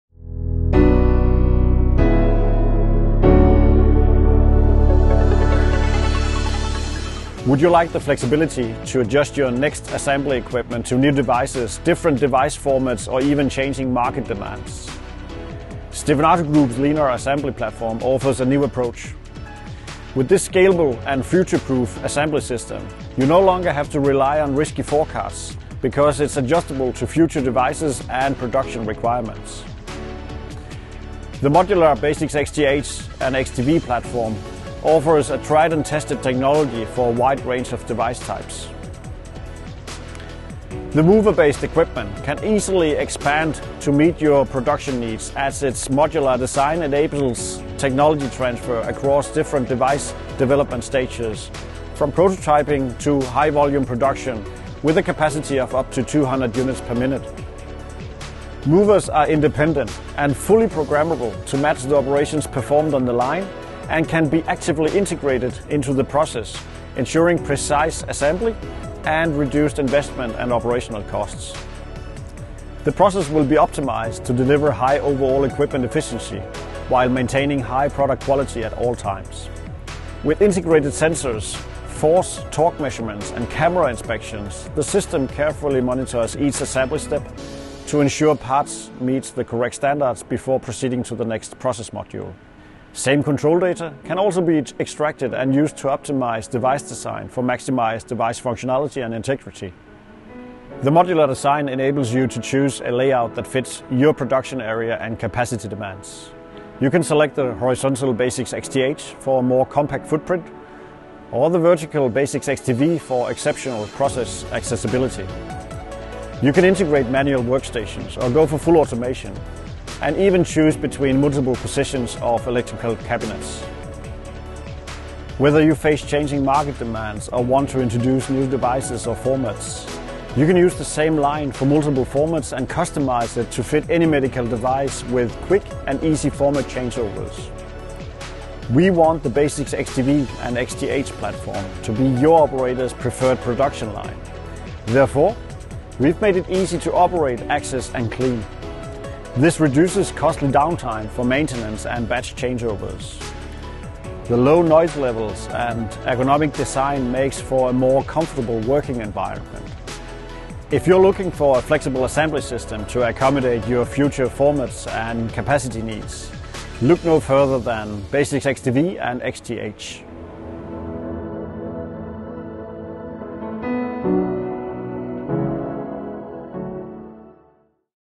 Linear Assembly Platform
The best-in-class choice for pharma companies and contract manufacturers to assemble pen injectors, auto-injectors and several medical devices from all sorts of life-science areas.